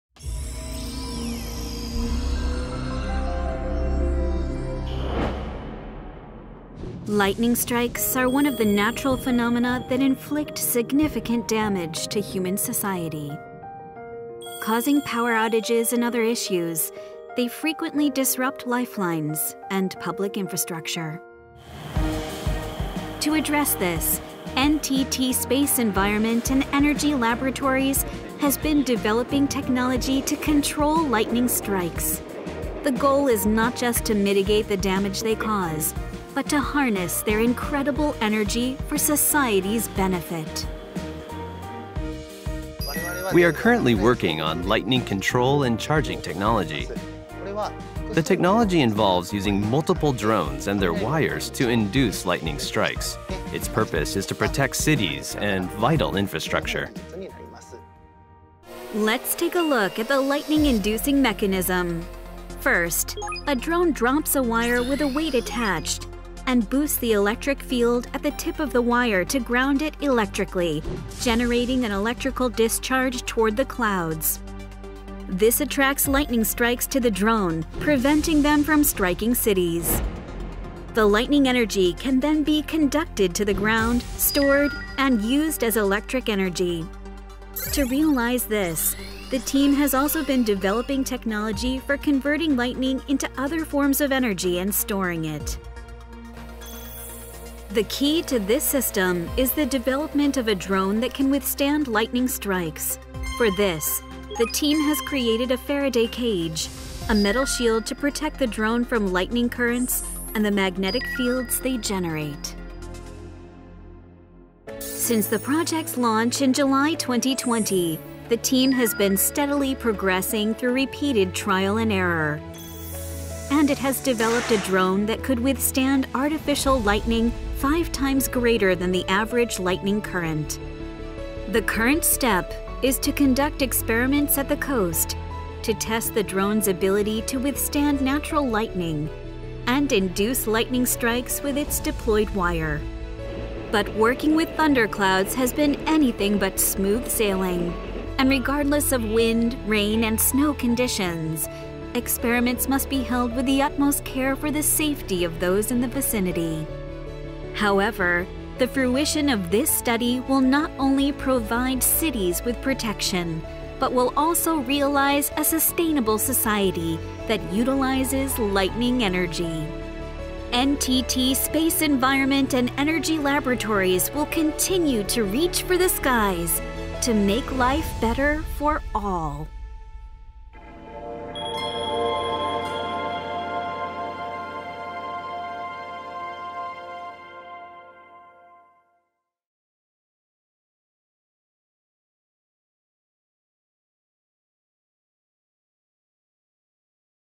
【NTT】Experiment of intentionally inducing lightning strikes using a lightning-resistant drone
【Lightning Control and Charging Technology】
Lightning strikes are one of the natural phenomena that inflict significant damage to human society.
Causing power outages and other issues, they frequently disrupt lifelines and public infrastructure.
To address this, NTT Space Environment and Energy Laboratories has been developing technology to control lightning strikes.
The goal is not just to mitigate the damage they cause but to harness their incredible energy for society’s benefit.